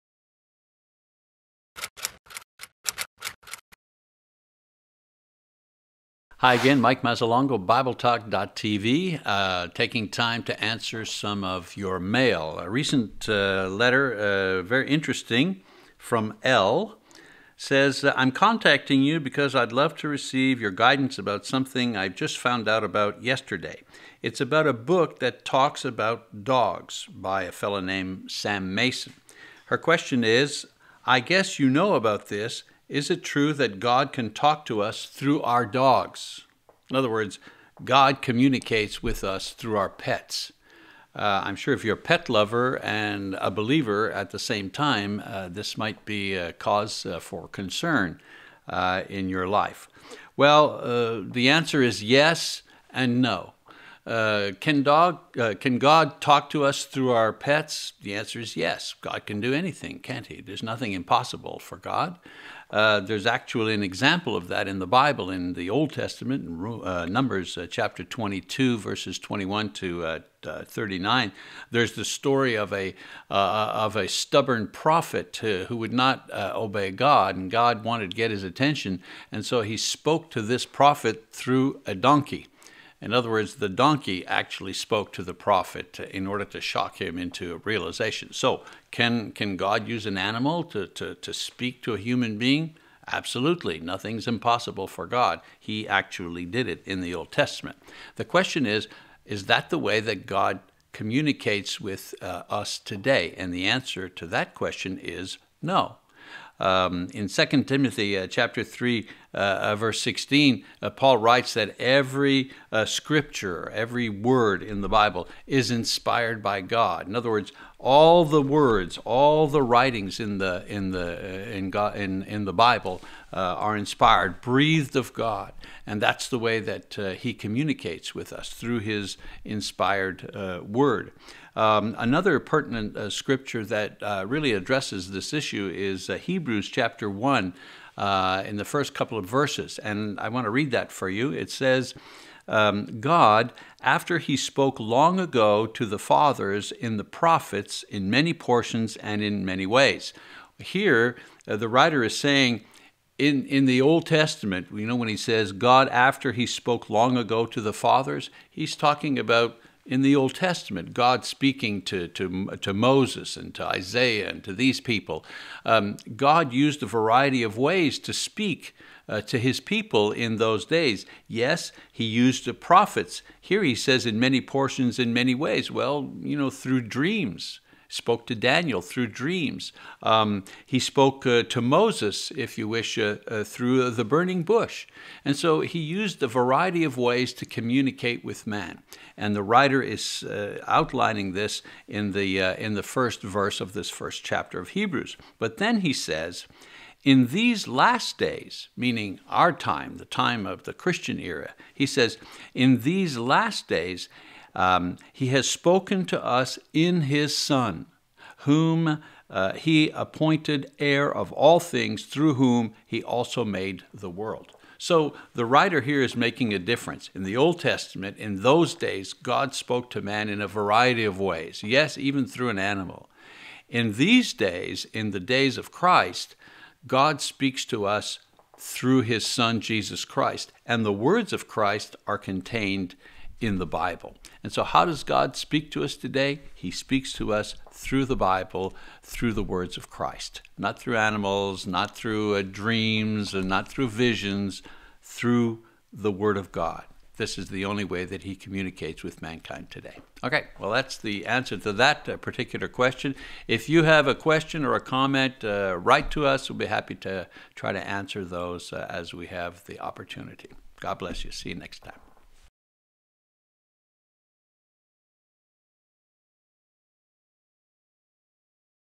Does God Speak Through Our Pets? | Mike Mazzalongo | BibleTalk.tv
Dear Mike,
I'm contacting you because I'd love to receive your guidance about something I've just learned about yesterday and it is about the book Talking Dogs by Sam Mason. Is it true that God can talk to us through our dogs?